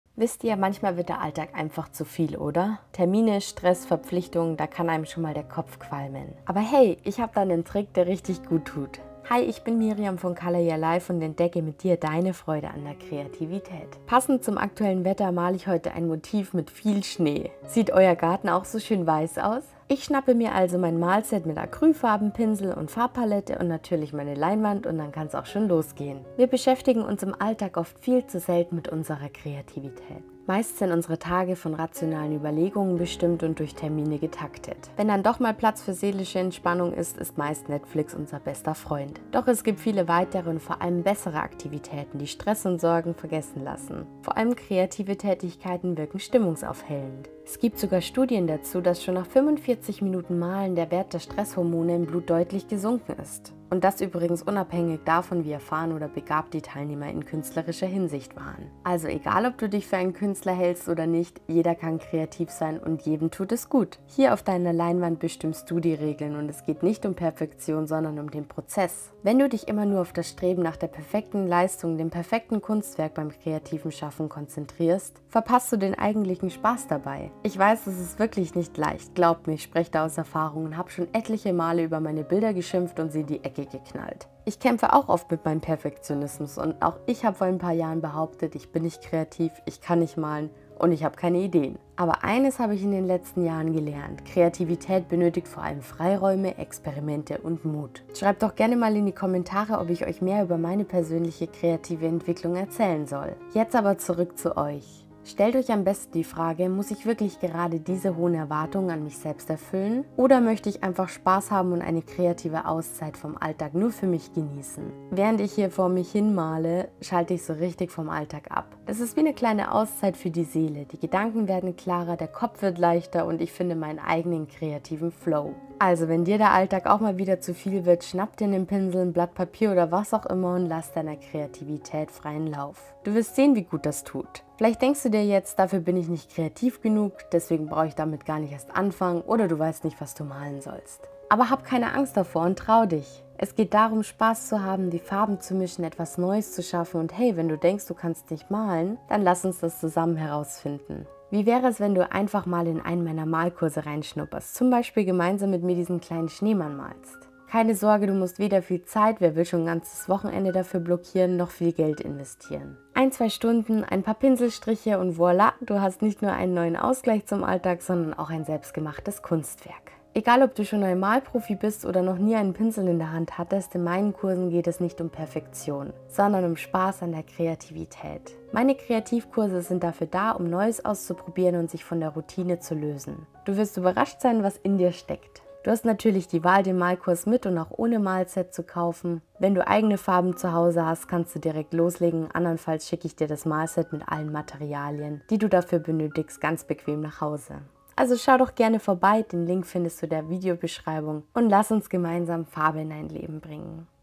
Acrylmalen für Anfänger: Wie male ich einen Schneemann? ⛄️
Viel Spaß beim Malen!